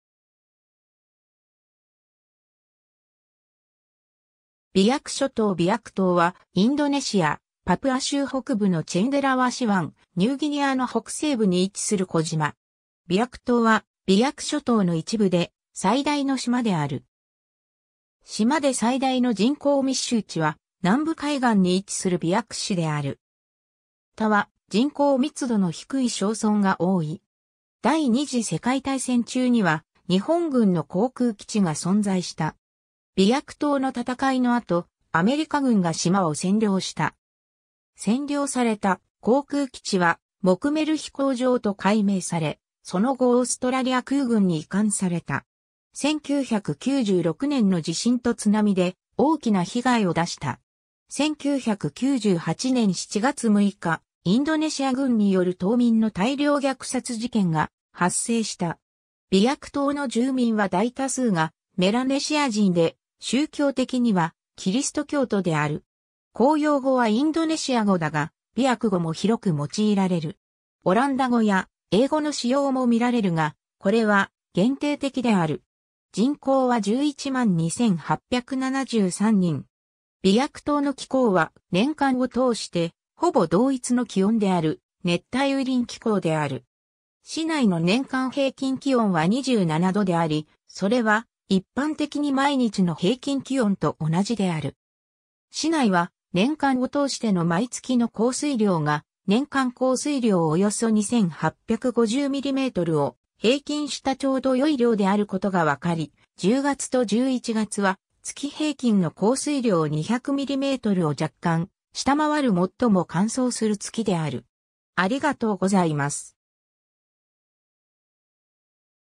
ビアク島
ビアク諸島 ビアク島（ビアクとう、インドネシア語: Pulau Biak）はインドネシア、パプア州北部のチェンデラワシ湾、ニューギニアの北西部に位置する小島。
ビアク島はビアク諸島の一部で、最大の島である。
 島で最大の人口密集地は南部海岸に位置するビアク市である。
他は人口密度の低い小村が多い。
 第二次世界大戦中には日本軍の航空基地が存在した。
ビアク島の戦いの後、アメリカ軍が島を占領した。
占領された航空基地は「モクメル飛行場」と改名され、その後オーストラリア空軍に移管された。
 1996年の地震と津波で大きな被害を出した。
 1998年7月6日、インドネシア軍による島民の大量虐殺事件が発生した。
 ビアク島の住民は大多数がメラネシア人で、宗教的にはキリスト教徒である。
公用語はインドネシア語だが、ビアク語も広く用いられる。
オランダ語や英語の使用もみられるが、これは限定的である。
人口は11万2873人（2010年国勢調査）。
 ビアク島の気候は、年間を通してほぼ同一の気温である熱帯雨林気候である。
市内の年間平均気温は27度であり、それは一般的に毎日の平均気温と同じである。
 市内は、年間を通しての毎月の降水量が年間降水量およそ2,850mmを平均したちょうど良い量であることがわかり、10月と11月は、月平均の降水量200mmを若干下回る最も乾燥する月である。